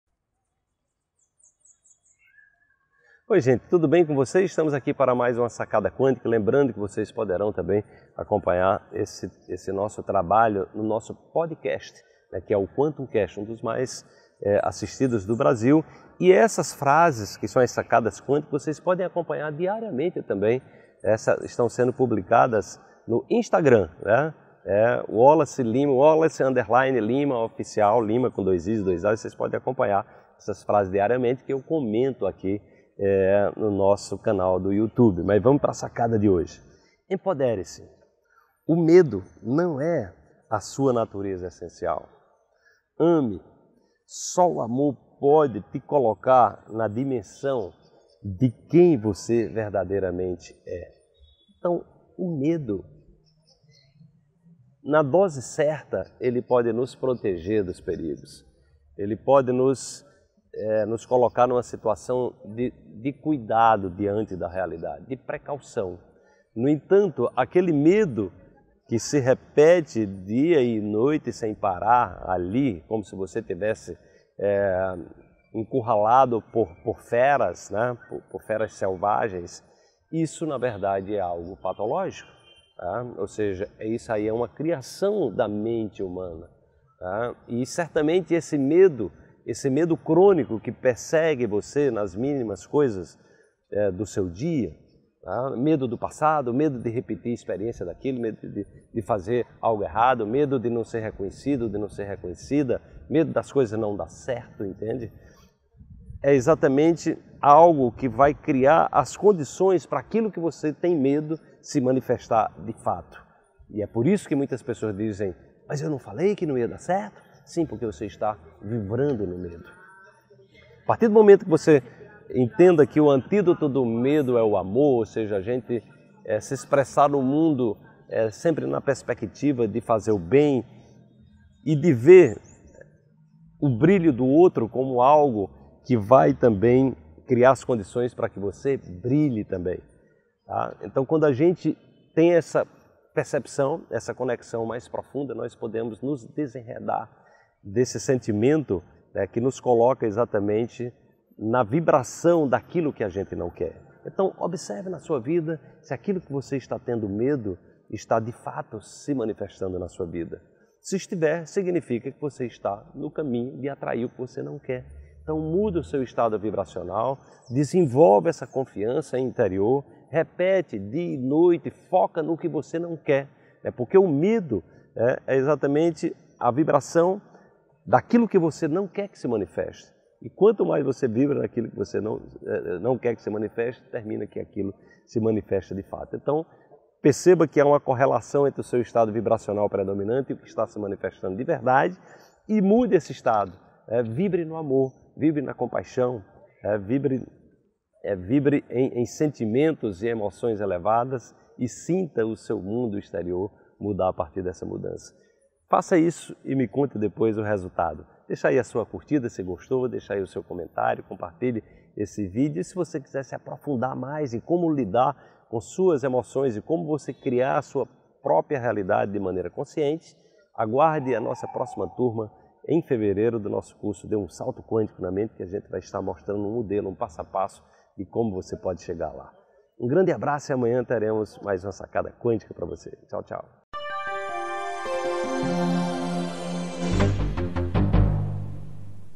Coaching Quântico 219: Encontre Sua Verdadeira Dimensão. Entenda Como! | Wallace Liimaa
✭✭✭✭✭ Nesse vídeo apresento dicas importantes para que você livre-se do medo crônico e manifeste seus desejos. Assista já!